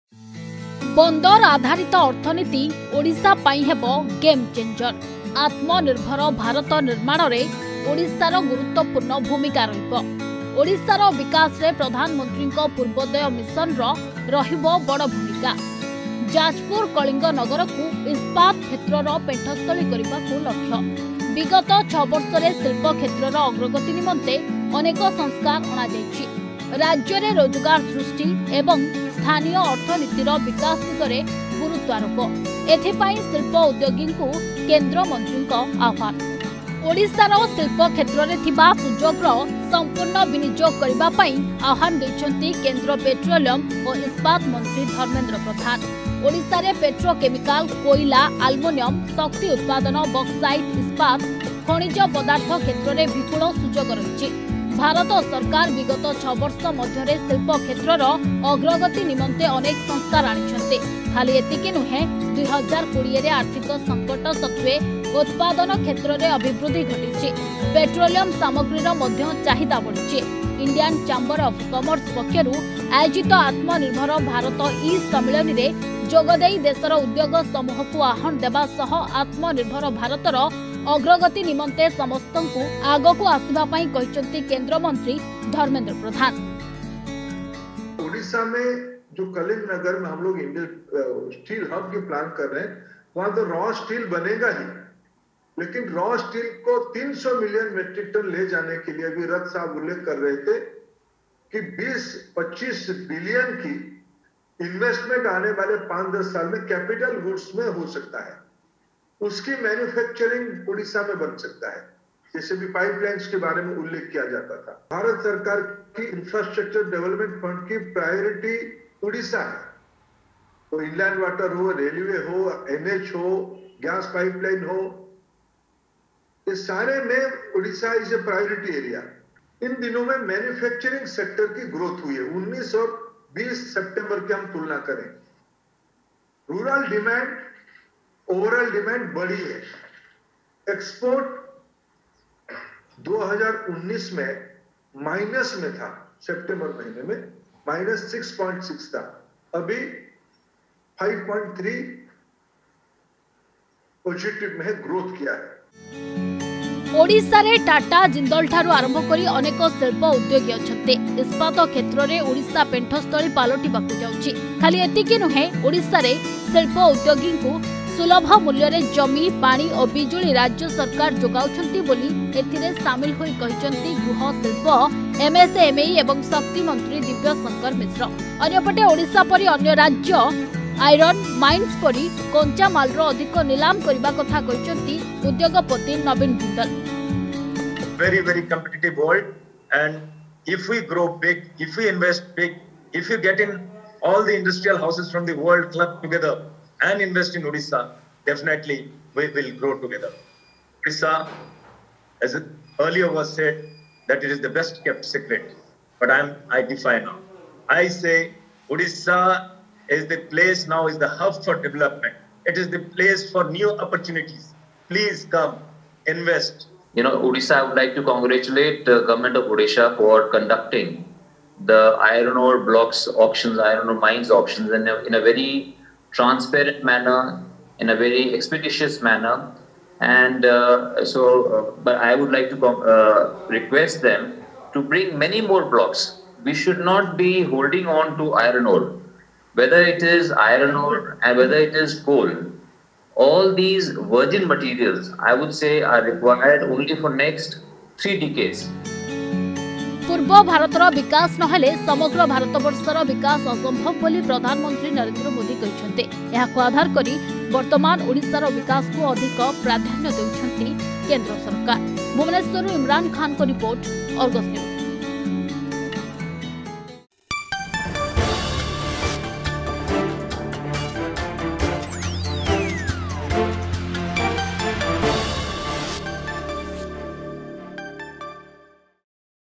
ବନ୍ଦର ଆଧାରିତ ଅର୍ଥନୀତି ଓଡ଼ିଶା ପାଇଁ ହେବ ଗେମଚେଞ୍ଜର
ଦେଶର ଉଦ୍ୟୋଗ ସମୂହକୁ କେନ୍ଦ୍ରମନ୍ତ୍ରୀଙ୍କୁ ଧର୍ମେନ୍ଦ୍ର ପ୍ରଧାନଙ୍କ ଆହ୍ୱାନ । ଓଡିଶା ଶିଳ୍ପ କ୍ଷେତ୍ରରେ ଥିବା ସୁଯୋଗର ସମ୍ପୂର୍ଣ୍ଣ ବିନିଯୋଗ କରିବା ପାଇଁ ଆହ୍ୱାନ ଦେଇଛନ୍ତି କେନ୍ଦ୍ରମନ୍ତ୍ରୀ ...